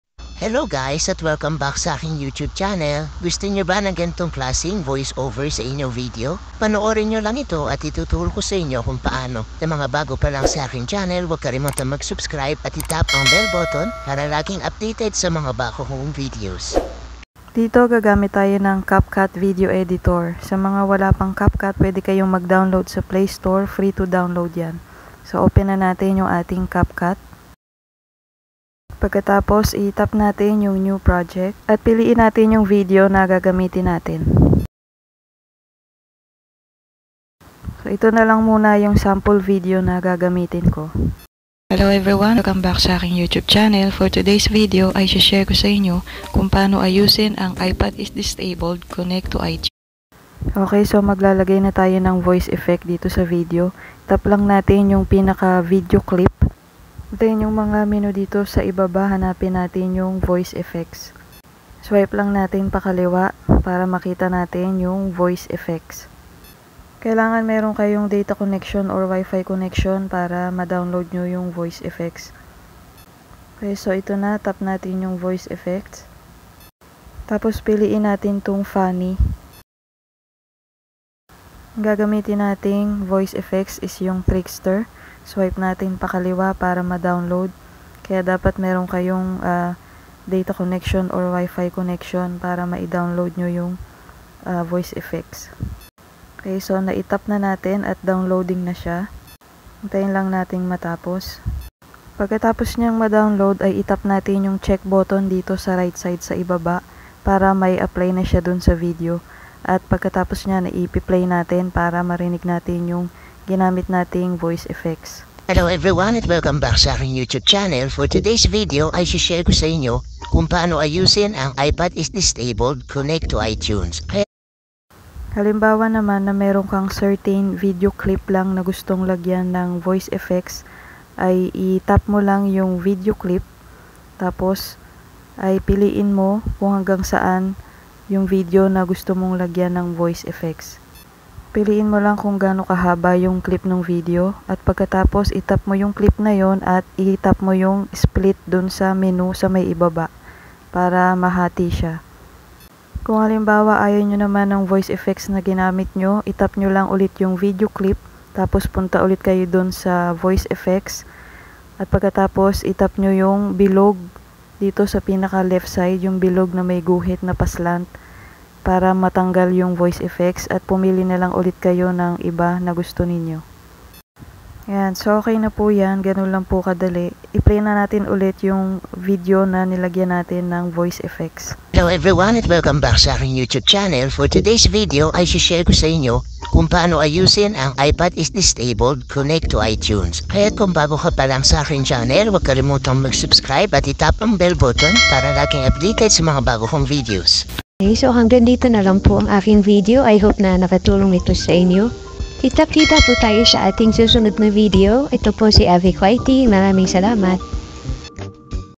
ITO ANG VOICE OVER NA TRENDING/PAANO GAWIN 2024
Good Day Every Juan!
Sa video na ito ay ituturo ko sainyo kung paano gawin ang voice over na trending at viral. Panoorin po ang step by step guide na ito. Maraming Salamat!